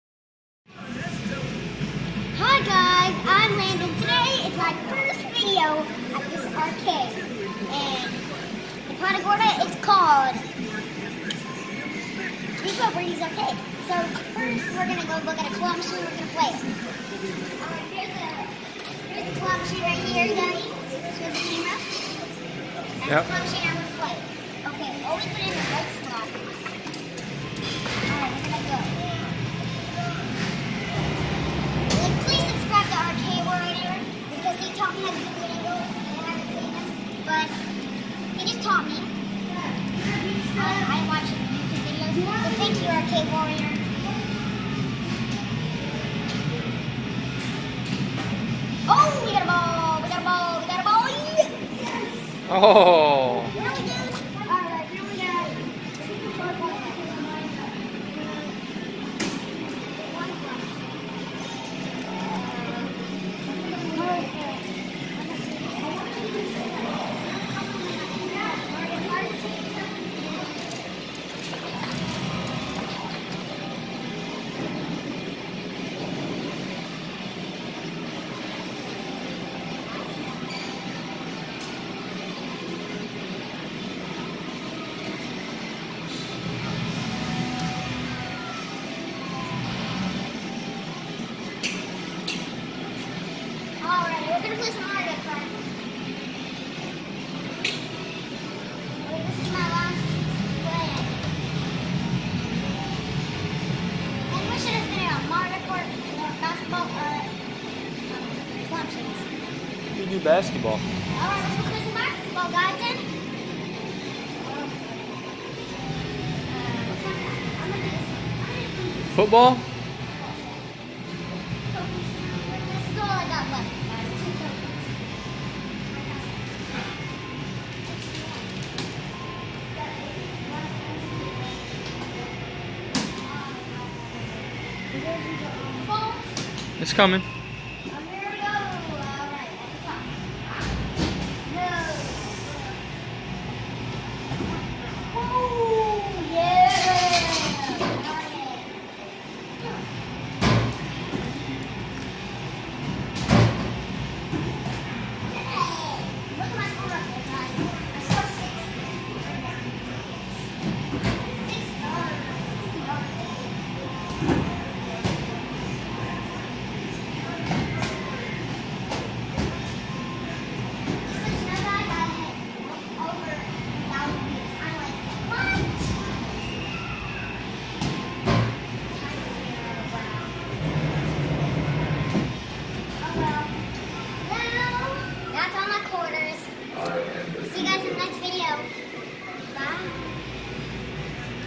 Arcade trip
Landon at the arcade playing a few games